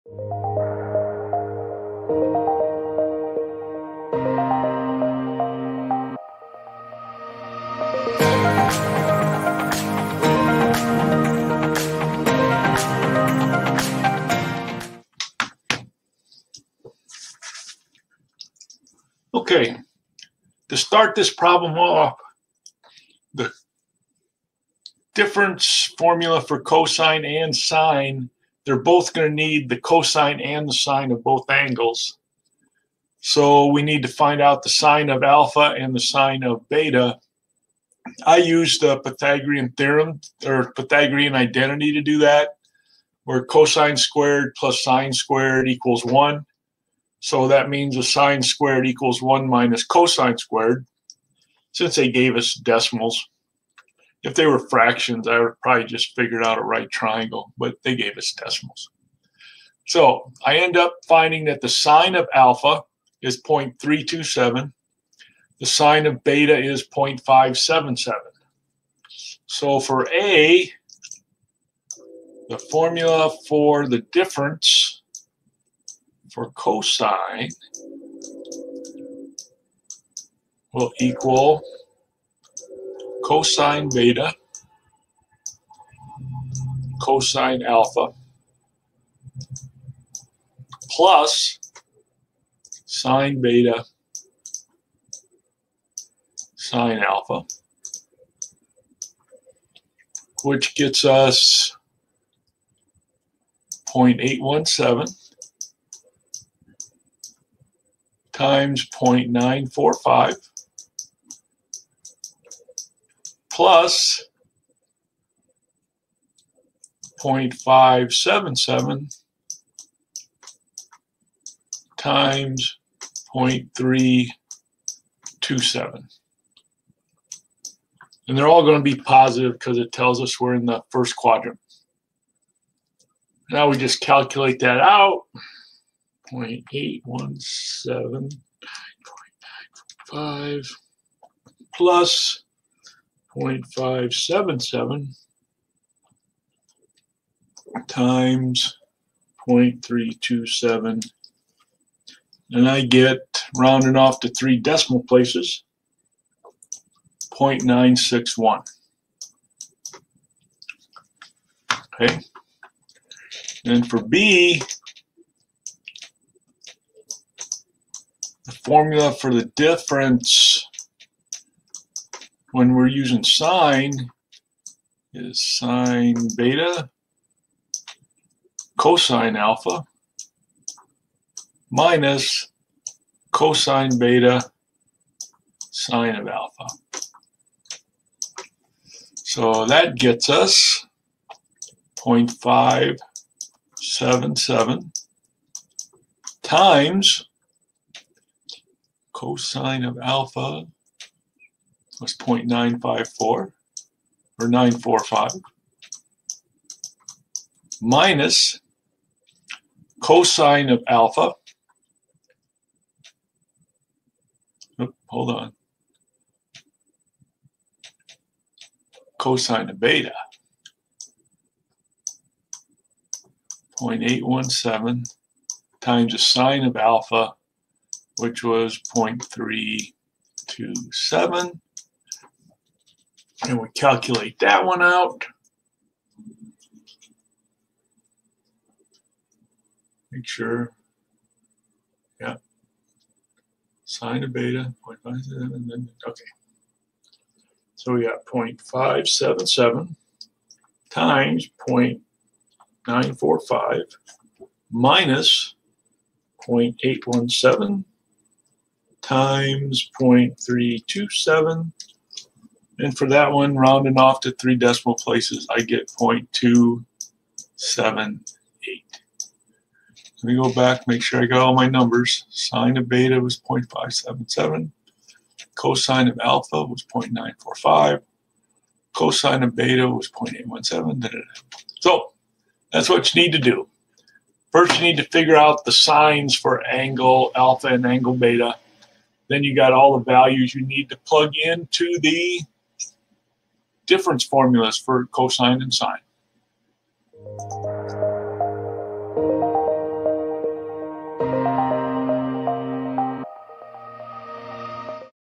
sin and cos angle difference formulas
Question: If     cos α = 0.945     and     cos β = 0.817     with both angles’ terminal rays in Quadrant-I, find the values of (a) cos ( β − α ) =     (b) sin ( β − α ) =

Answered By:

James C.